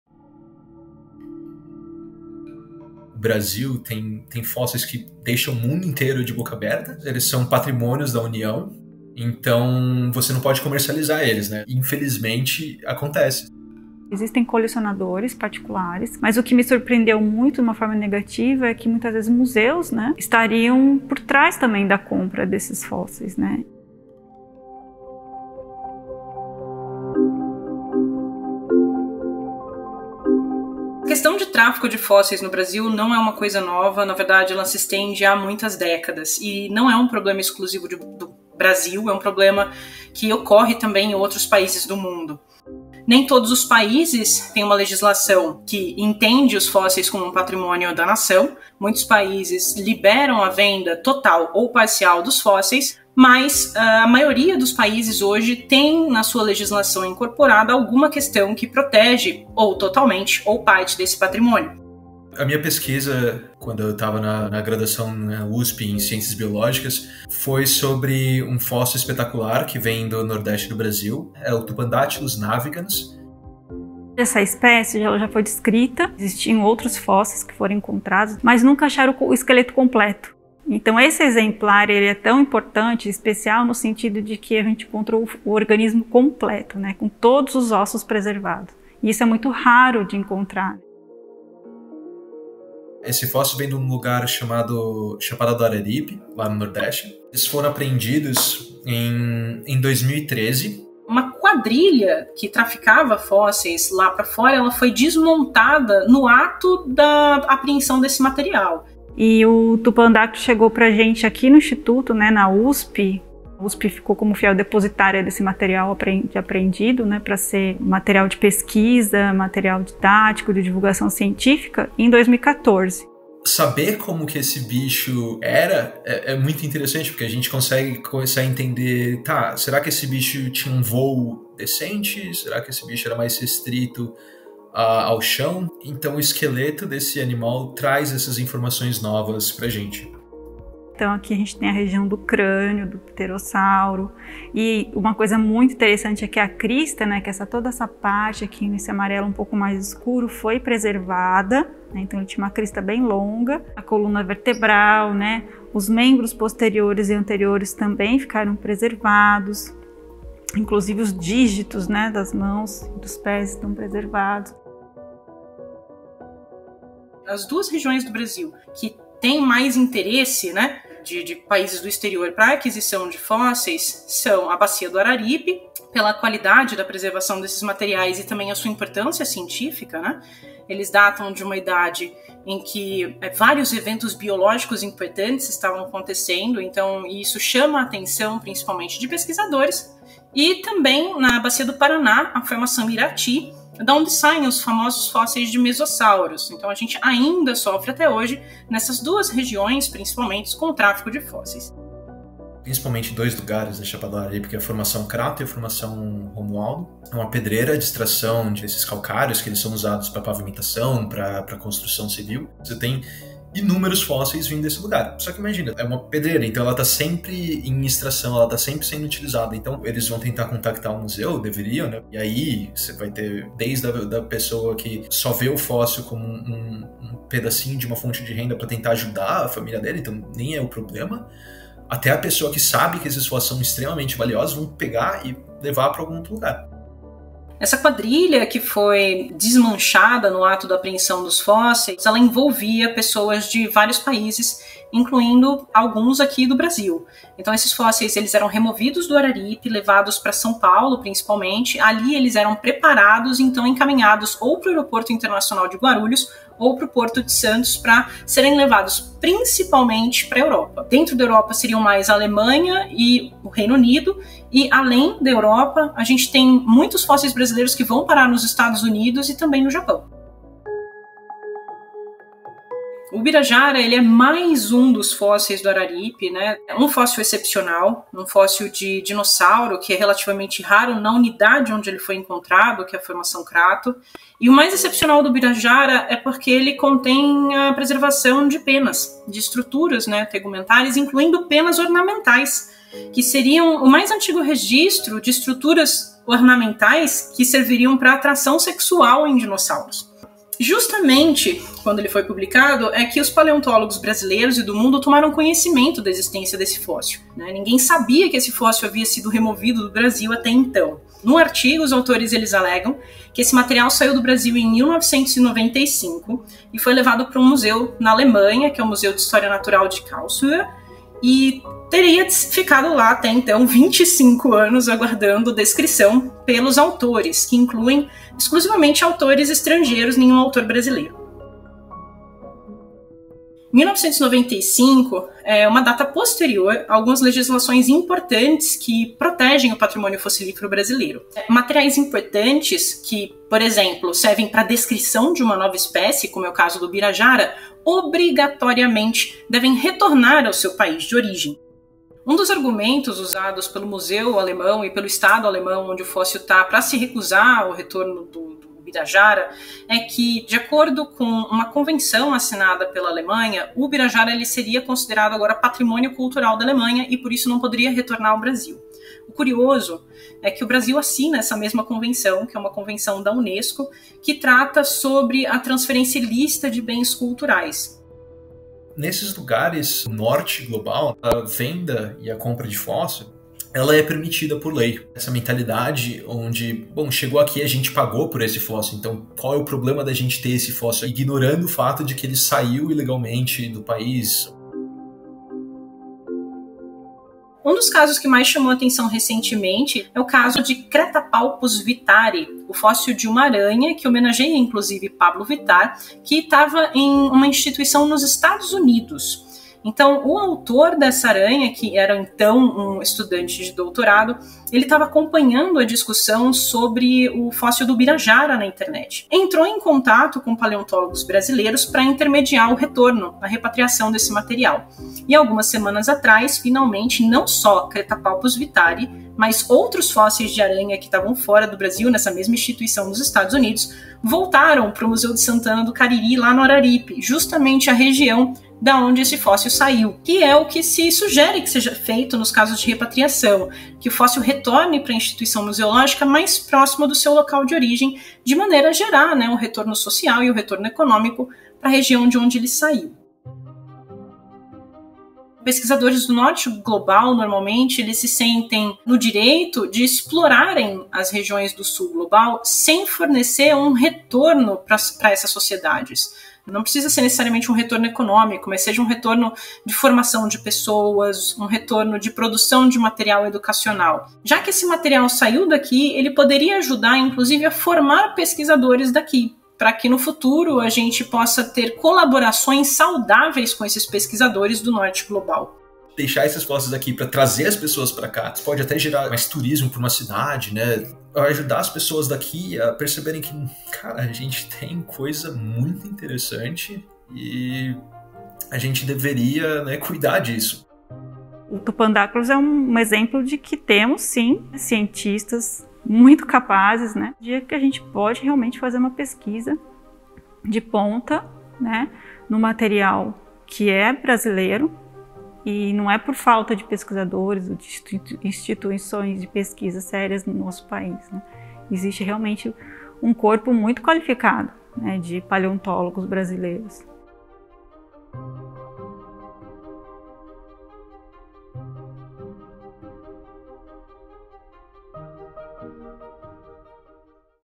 O problema do contrabando dos fósseis
Paleontólogos se articulam com o Ministério Público e a Polícia Federal para frear o tráfico de patrimônio fossilífero no Brasil.